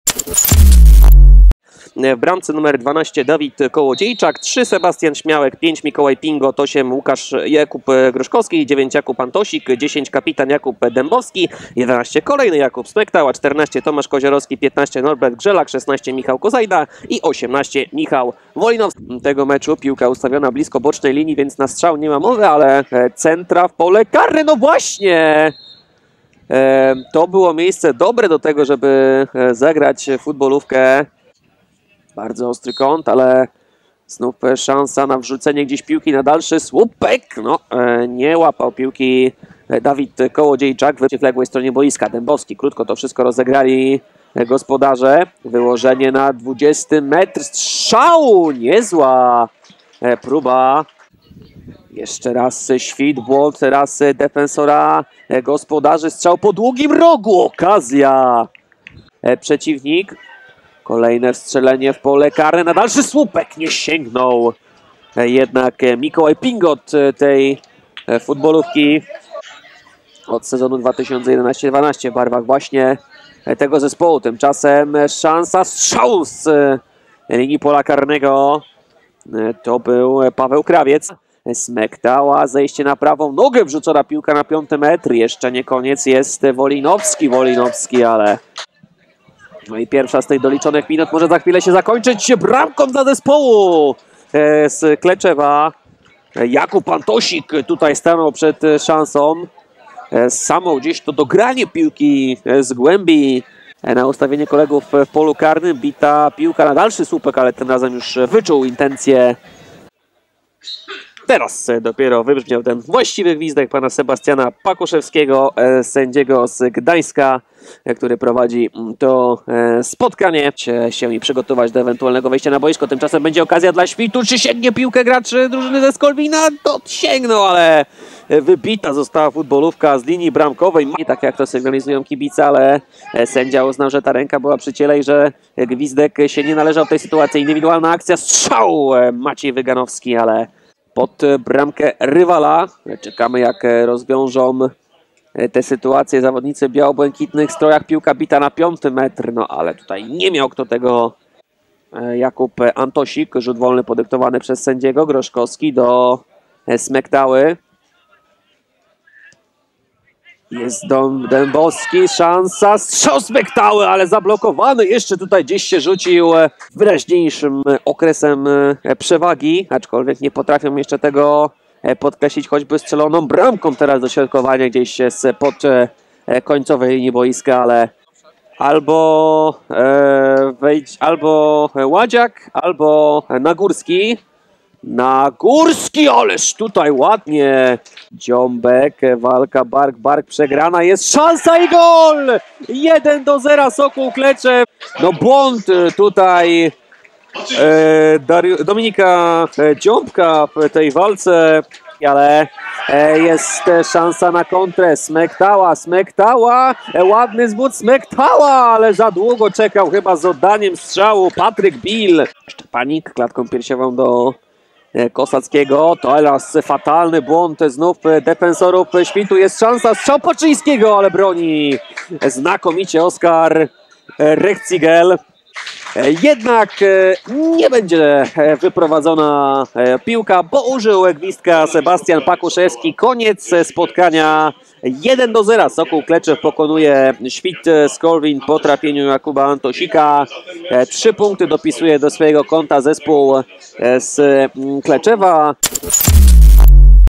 Sokół Kleczew - Świt Skolwin 1:0 / SKRÓT / III liga gr. 2 [13.10.2018]
Zobacz najciekawsze akcje z meczu 3 ligi piłki nożnej Sokół Kleczew - Świt Skolwin z KP Sport